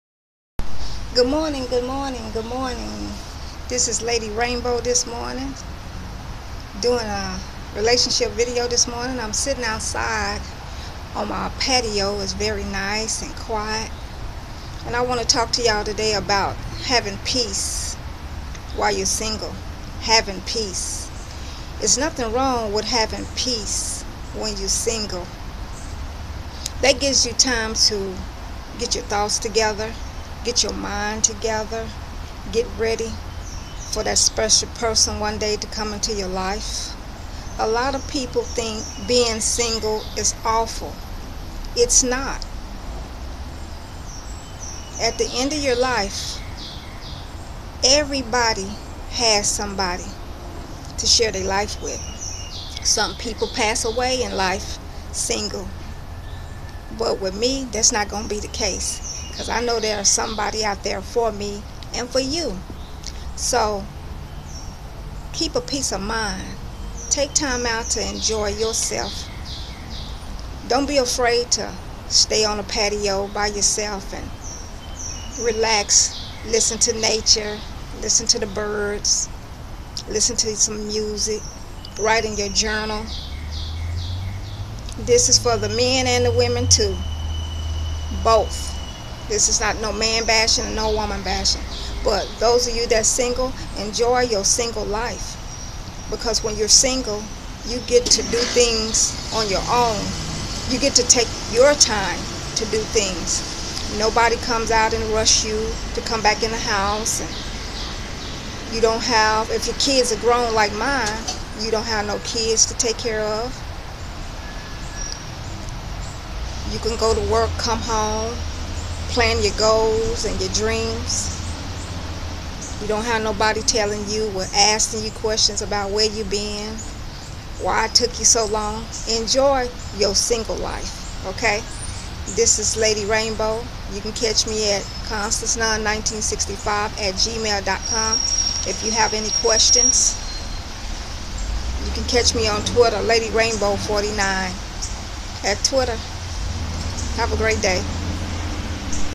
Relationships/How to have peace
Its nothing like having a peace of mind, even Christ had to take time out from his disciples just to have peace. That's what we should take time out to do. When you are single you have all the time to spend time with yourself. Enjoy the single life because you can do things, and you don't have anyone following behind you.
Asking you where you are going. Take time out for you.

ladyrainbow49@twitter